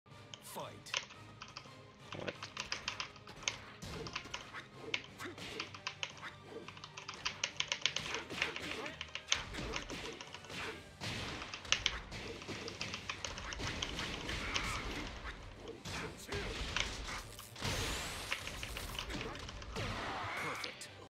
b3 combo after db2 charged at the wall and if they hold back df3+4 is guaranteed 🥶🥶🥶
You can also let a little comment under the video 🤗